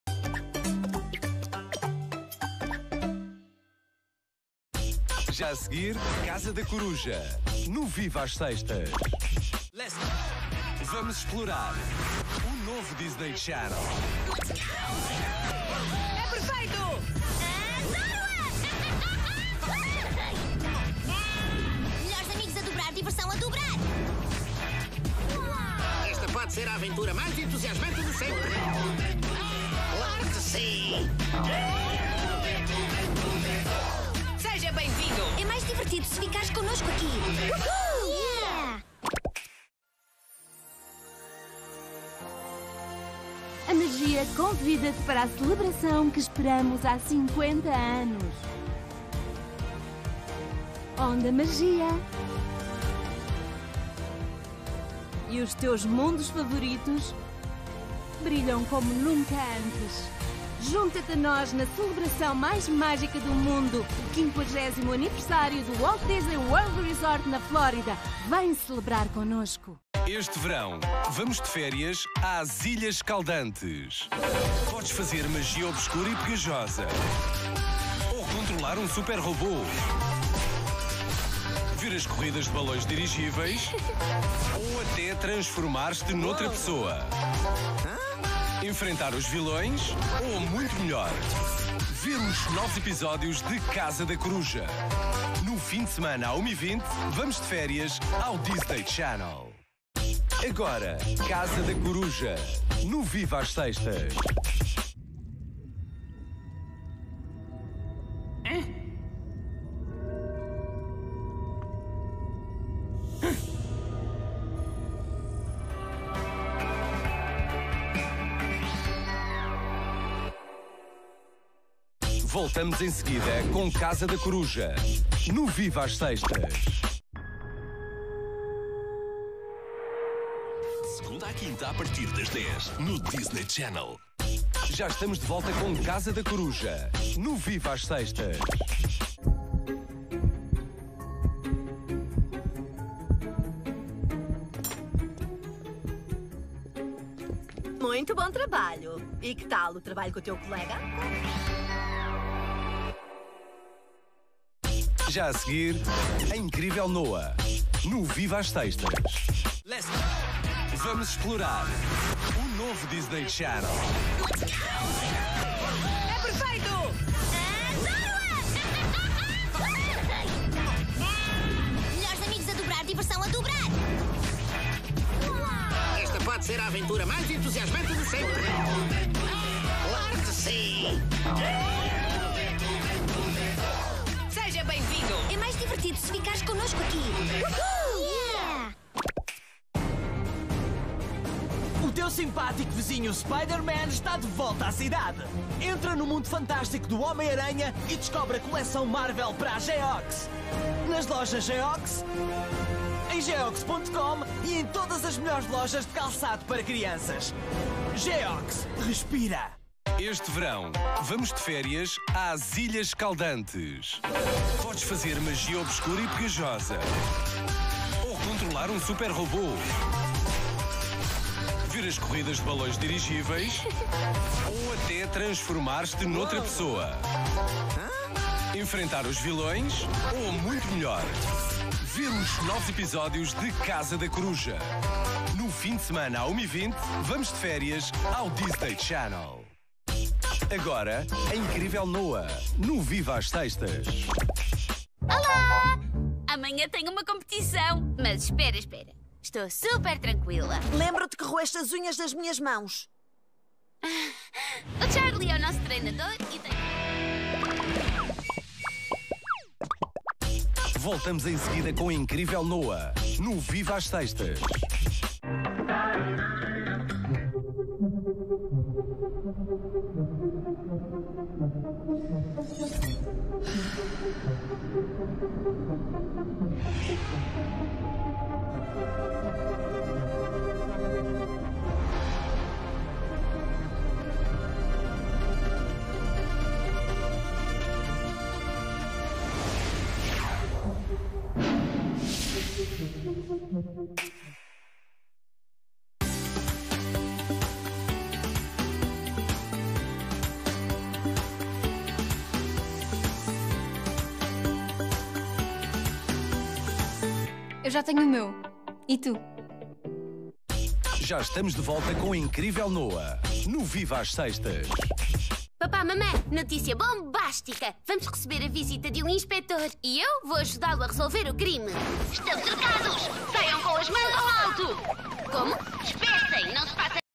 Disney Channel (Portugal) Publicidade/Continuity - 26/08/2022 (2)
O segundo bloco de publicidade foi omitido devido a incluir um trailer para Avatar: O Caminho da Água, que pode ser visto , e o anúncio das Noites de Verão já ter sido gravado vezes suficientes ao longo dos últimos dois meses.